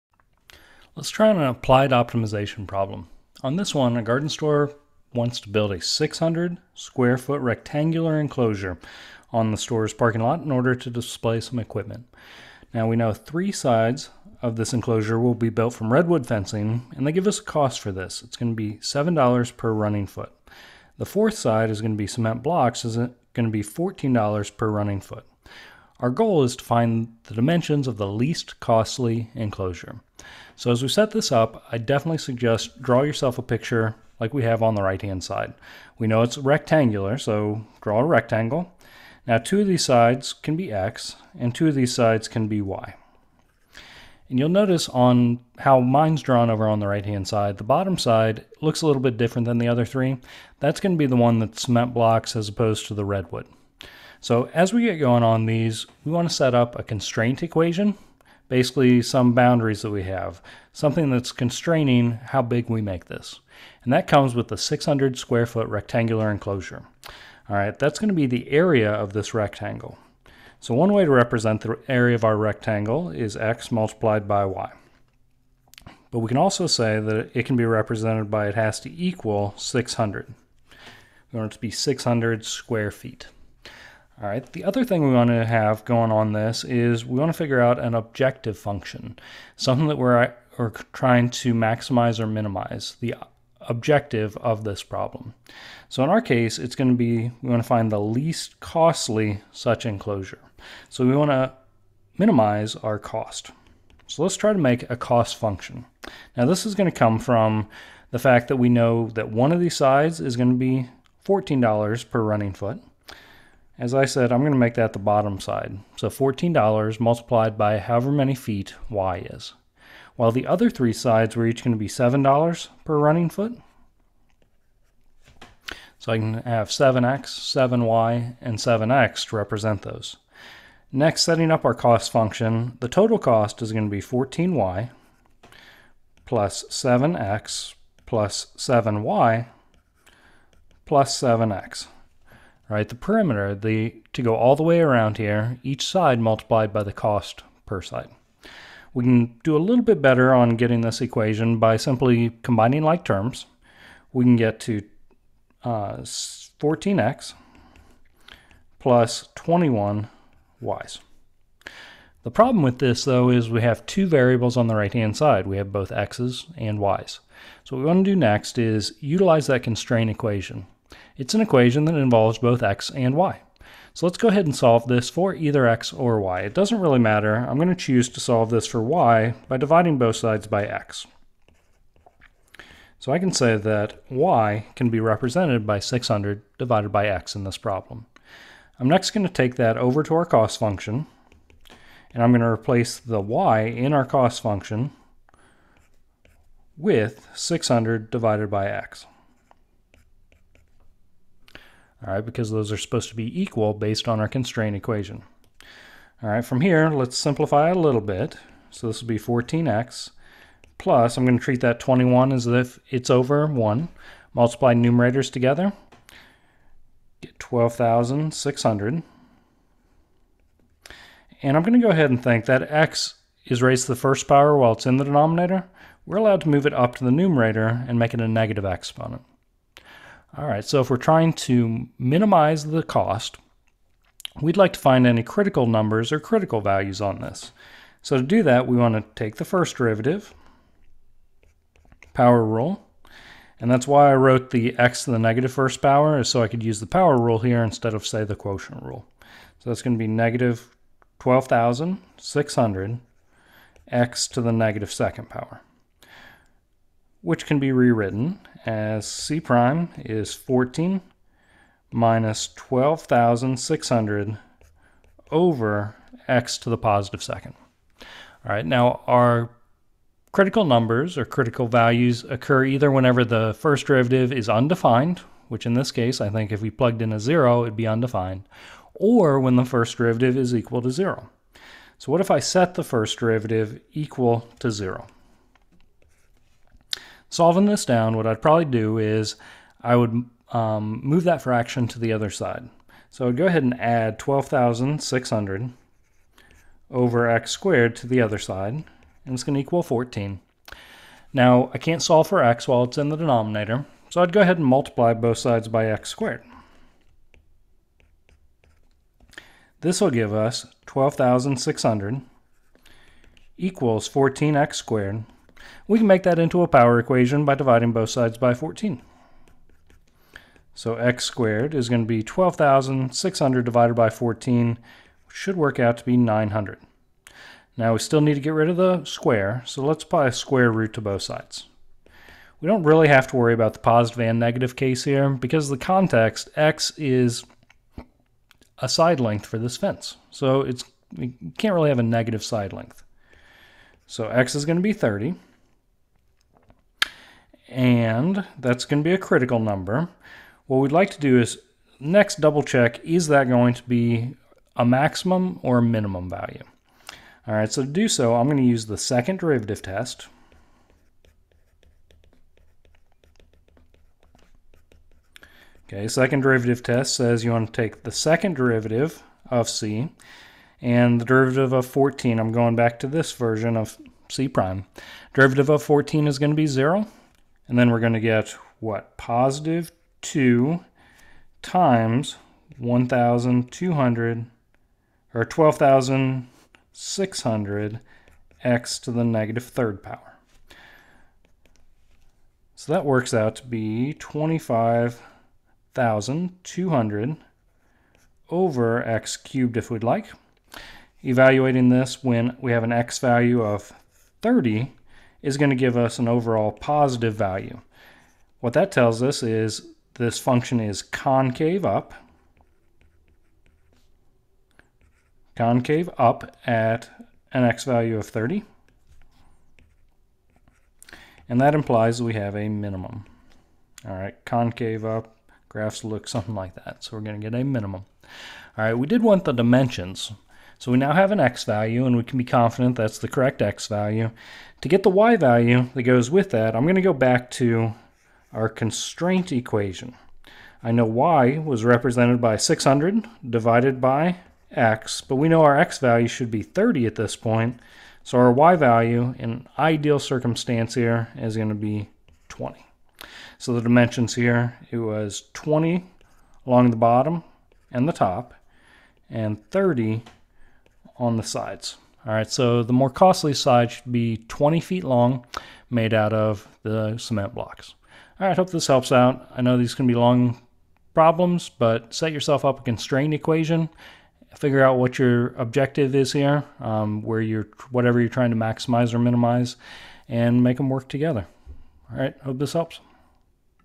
Applied Optimization | Minimize Fencing Cost
In this example problem, we work an applied optimization word problem by drawing a picture of the situation, setting up a constraint equation, and objective function.  In this case, the constraint equation is that we are limited in the area that we want to enclose with our rectangular fence.  The objective is to minimize the cost of our fence, so we set up a cost function using two variables.  To get a cost function that only has one variable, we solve the constraint equation for one of the variables and substitute it into the objective.  We then take the derivative of the objective function, set it equal to zero, and solve to find a critical number (value).  We test if this will be a maximum or minimum at this critical point by using the 2nd derivative test.  After we confirm that it will be a minimum, we find the other dimension by substituting back into the constraint equation.